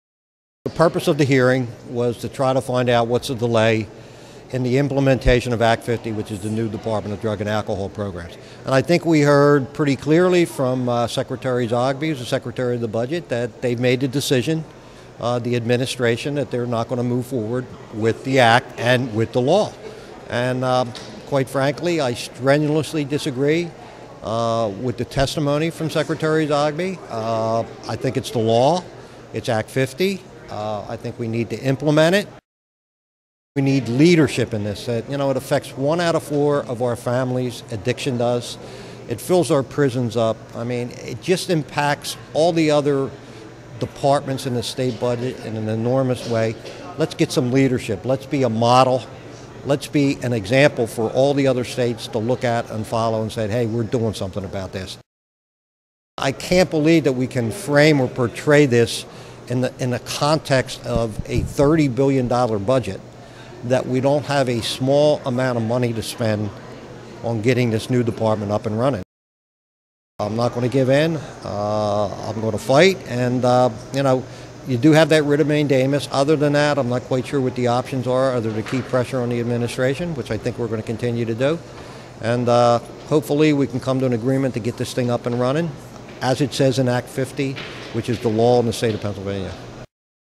Act 50 Deserves Implementation Without Delay
Rep. Gene DiGirolamo comments following a hearing on the administration's delay of implementing a new Department of Drug and Alcohol Programs.